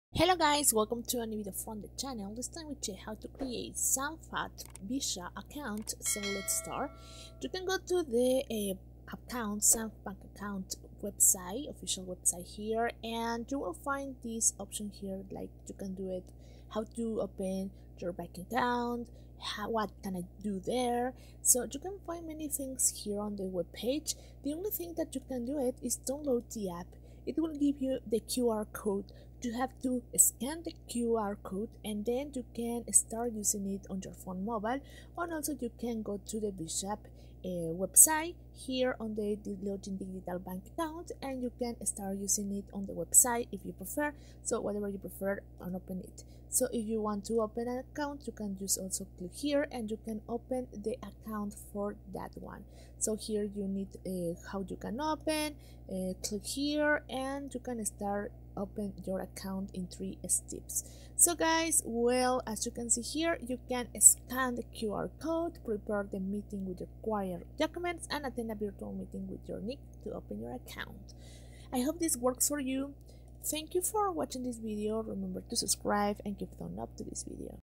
How to Create Sampath Vishwa Account (2025)
In this video, I'll show you How to Register for Sampath Vishwa Online Banking via a Live Bank Officer. The method is very simple and clearly described in the video. Follow all of the steps & How to Create Sampath Vishwa Account online.